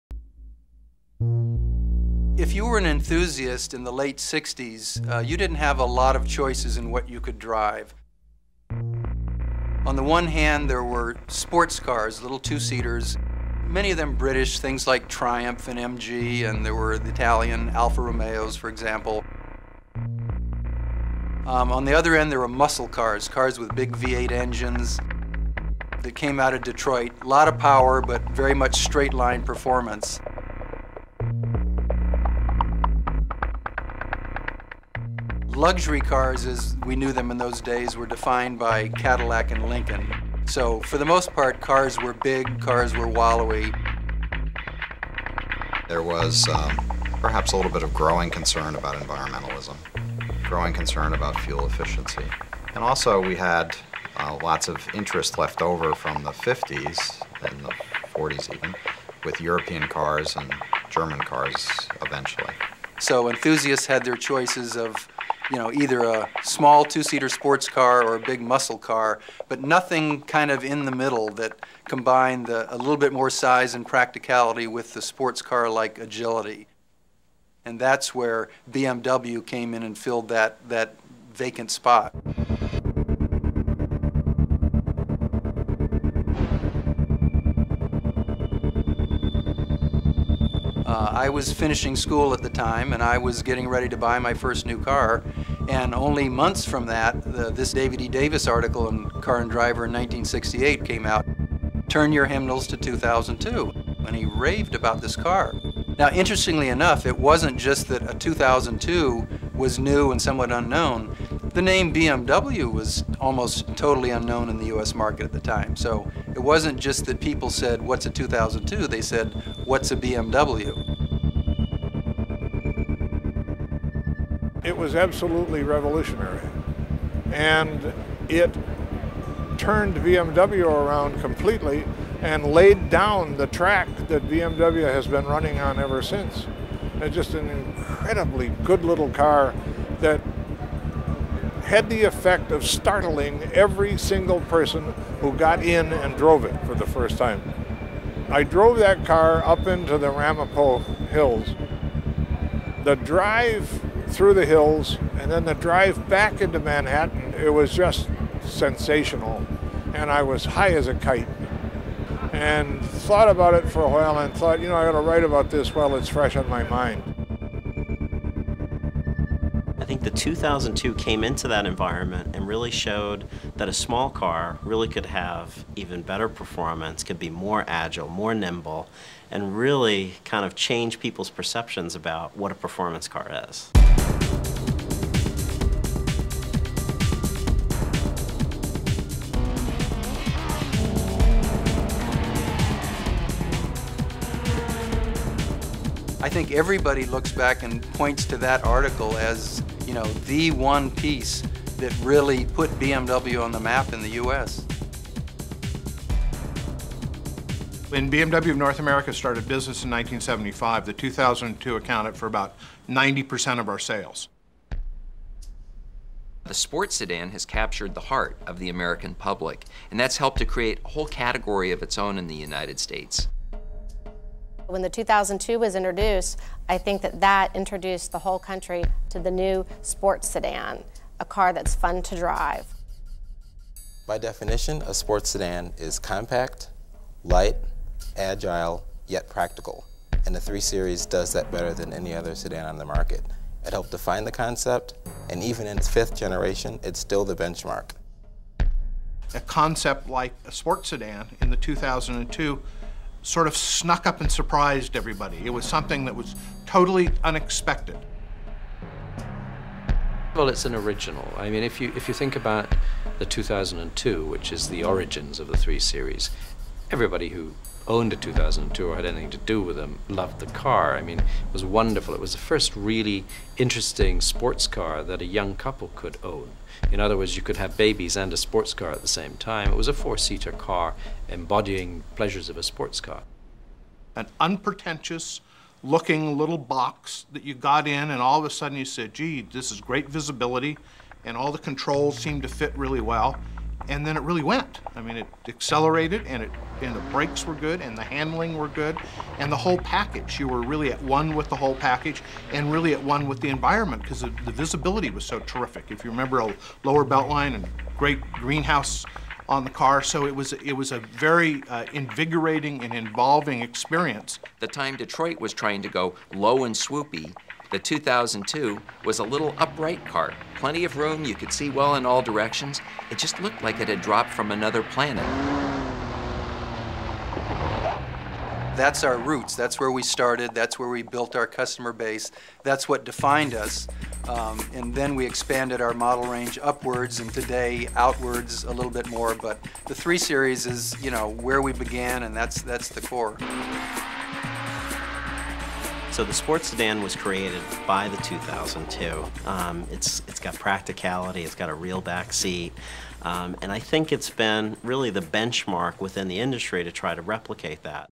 BMW 3 Series Heritage
Take a look back at how the 3 Series changed car history.